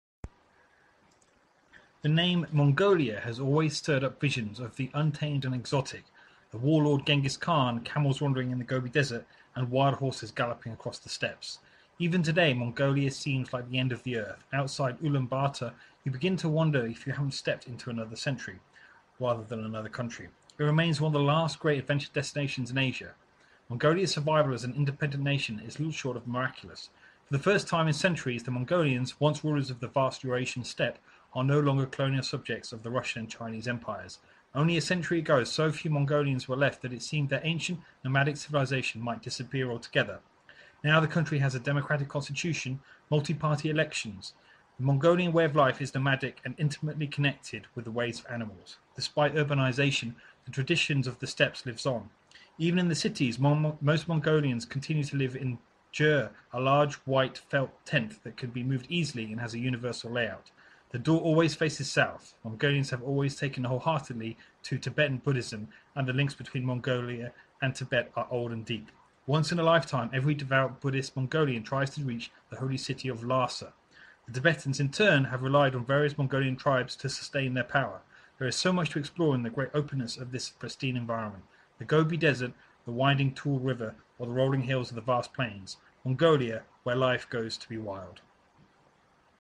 Mongolia
The name 'Mongolia' has always stirred up visions of the untamed and
exotic- the warlord Genghis Khan, camels wandering in the Gobi Desert
and wild horses galloping across the steppes. Even today, Mongolia seems
like the end of the earth - outside Ulaanbaatar you begin to wonder if
you haven't stepped into another century, rather than another country.
It remains one of the last great adventure destinations in Asia.

Mongolia's survival as an independent nation is little short of
miraculous. For the first time in centuries the Mongolians, once rulers
of the vast Eurasian steppe, are no longer colonial subjects of the
Russian and Chinese empires. Only a century ago, so few Mongolians were
left that it seemed their ancient, nomadic civilization might disappear
altogether. Now the country has a democratic constitution, multiparty
elections.

The Mongolian way of life is nomadic and intimately connected with the
ways of animals. Despite urbanization, the traditionss of the steppes
live on. Even in the cities, most Mongolians continue to live in a ger,
a large, white felt tent that can be moved easily and has a universal
layout: the door always faces south;

Mongolians have always taken wholeheartedly to Tibetan Buddhism and the
links between Mongolia and Tibet are old and deep. Once in a lifetime,
every devout Buddhist Mongolian tries to reach the holy city of Lhasa;
the Tibetans in turn have relied on various Mongolian tribes to sustain
their power.

There is so much to explore in the great openness of this pristine
environment. The Gobi Desert, the winding Tuul River, or the rolling
hills of the vast plains. Mongolia, where life goes to be wild.